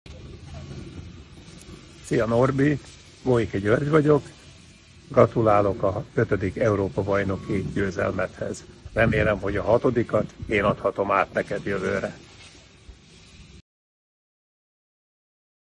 Goike György gratulál Kiss Norbi 5. Eb címéhez
Goike György, a Hungarocamion egykori versenyzője gratulál Kiss Norbi ötödik Kamion Eb bajnoki címéhez.